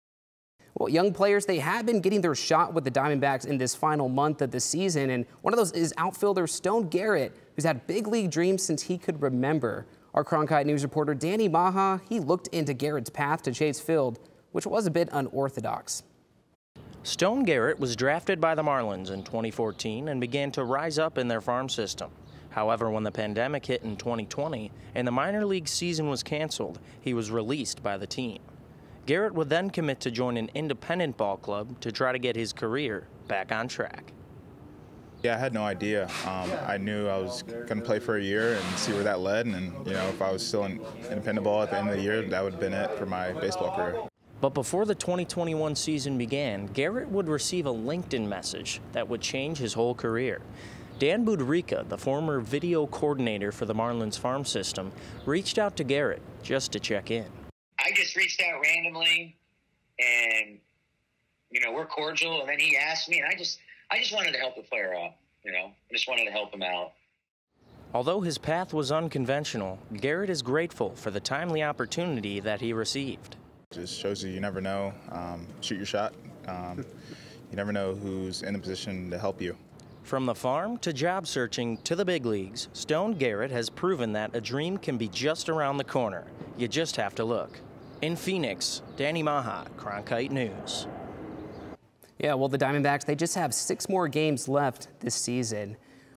Dbacks Stone Garrett
Diamondbacks outfielder Stone Garrett took an unconventional path to the major leagues. (Video by Daniel Machaj/Cronkite News)